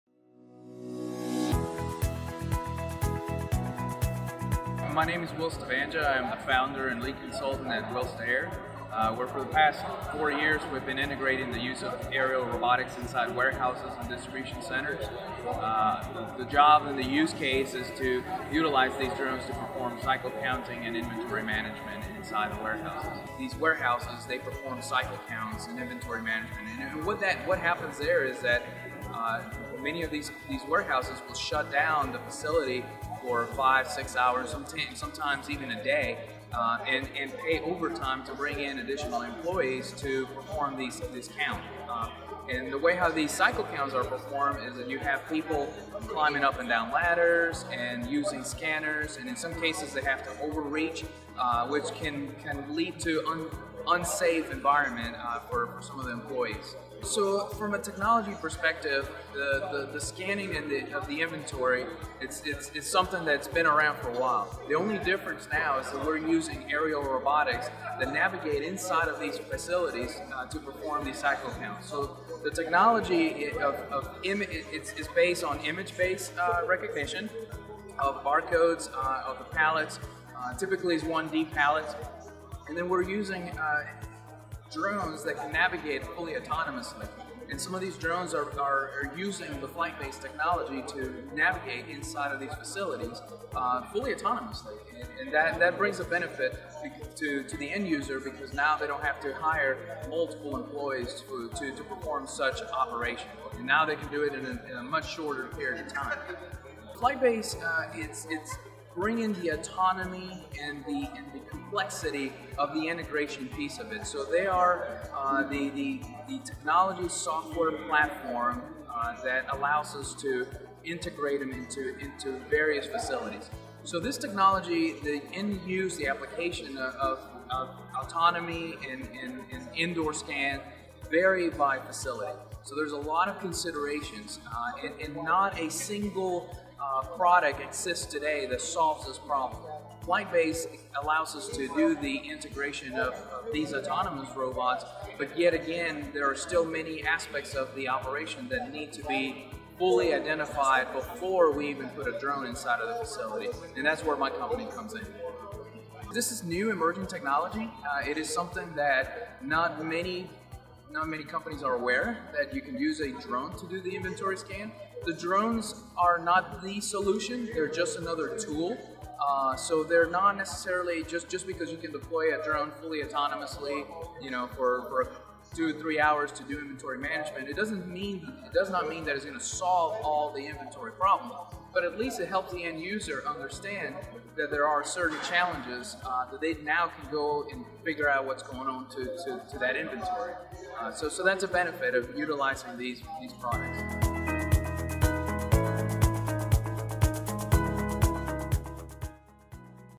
Autonomous Drones for Warehouse Management | Supply Chain | Cycle Counting
Will Stavanja, Founder, Wilstair, LLC shares insights on drone automation for inventory management in warehouses. FlytBase and Wilstair have partnered to help supply chain & logistics companies deploy autonomous drone fleets across their warehouses. Drones are creating immediate business value for automated cycle counting, inventory audit, inventory reconciliation, item search, anomalies detection, security & surveillance, roof inspection, and many other use-cases.

Download the white paper on drone automation for warehouse 4.0